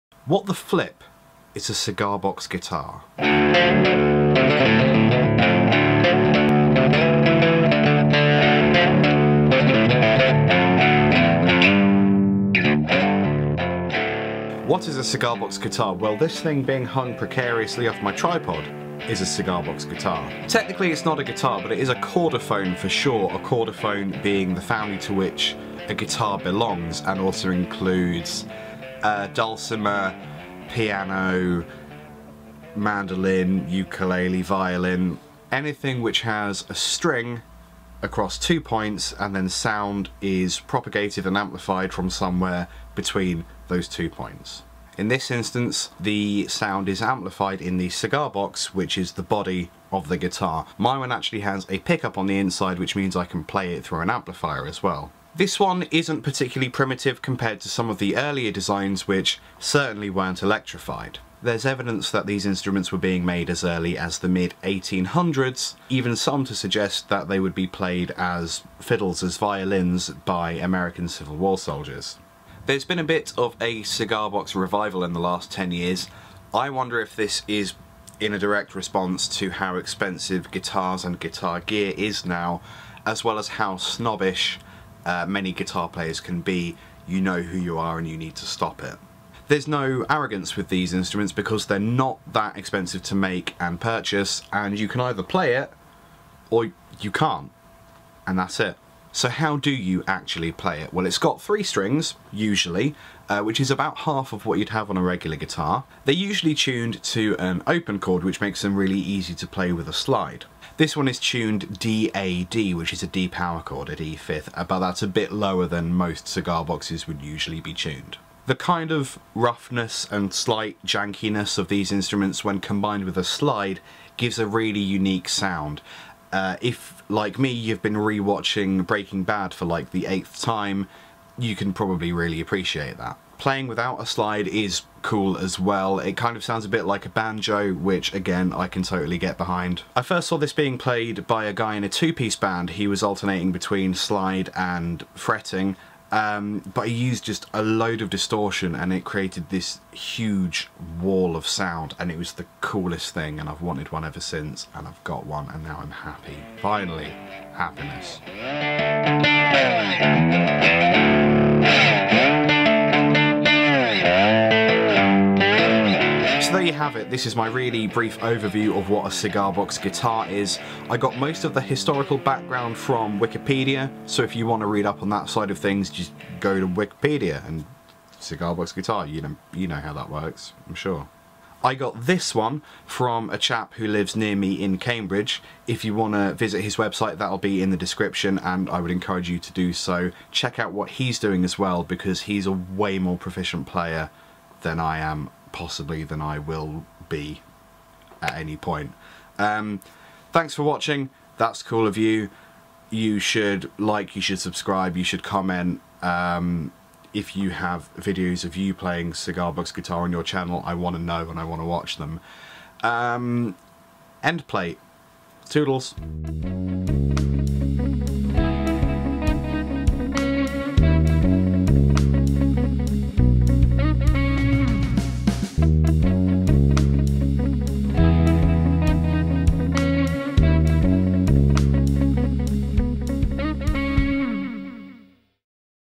WTF is a Cigar Box Guitar?
What even is a Cigar Box guitar? Well, let's find out together!